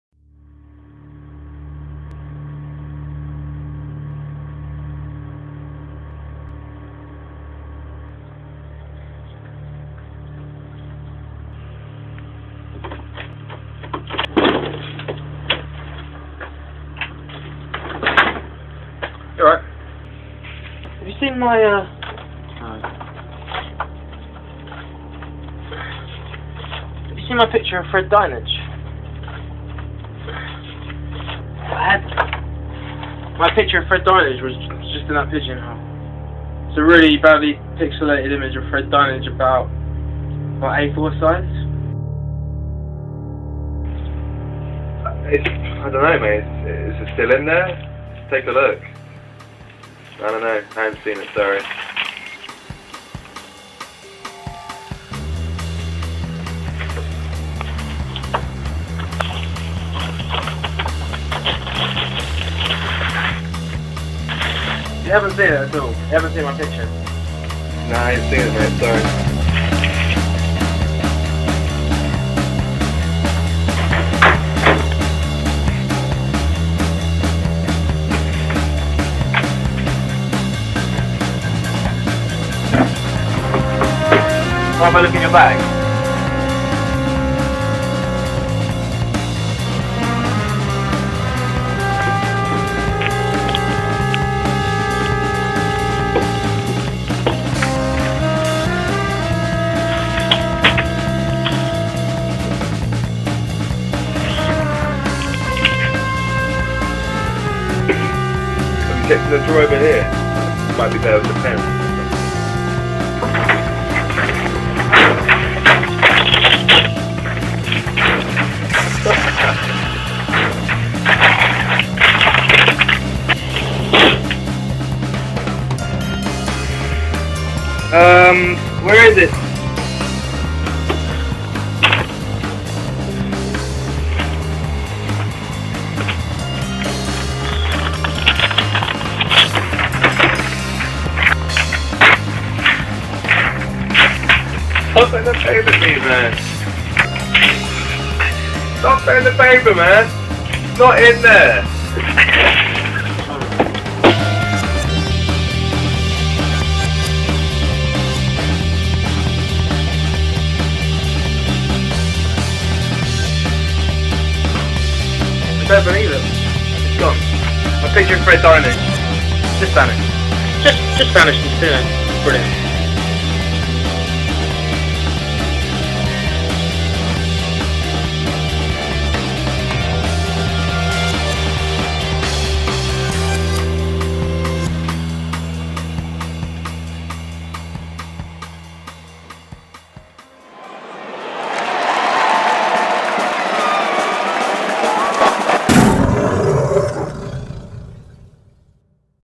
Fred Dinenage: The Movie
Former How 2 presenter and thinking man's Pat Sharp, Fred Dinenage,  embarks on a Tolkien-esque adventure to the frontiers of discovery and beyond. Soundtrack by Primal Scream. Includes strobing images which some will find unsettling.